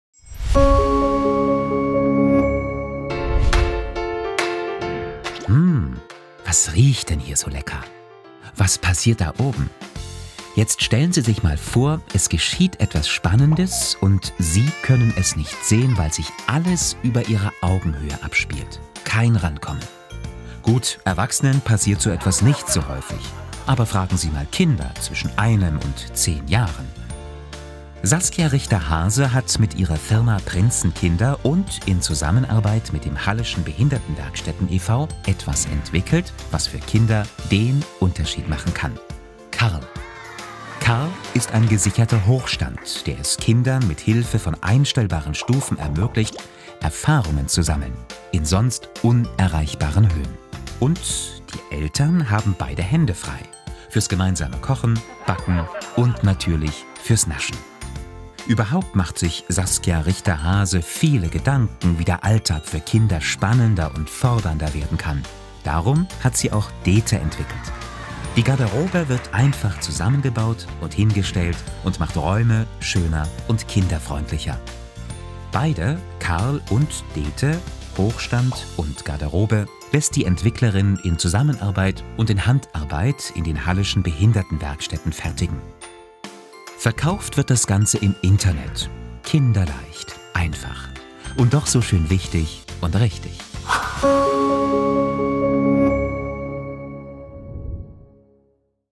Bestform Award 2017: Prinzenkinder
DER BESTFORM /// MEHR /// WERT /// AWARD WÜRDIGT GEMEINSAME ENTWICKLUNGEN VON KREATIVEN UND WIRTSCHAFTSPARTNERN ANDERER BRANCHEN. Im Mittelpunkt stehen die Zusammenarbeit und die Idee. Ziel ist es, zu zeigen, welches Potenzial in Sachsen-Anhalts Kreativwirtschaft steckt.

Dies sind die Preisträgerportraits 2017.

Produktionszeitraum: Mai-Juni 2017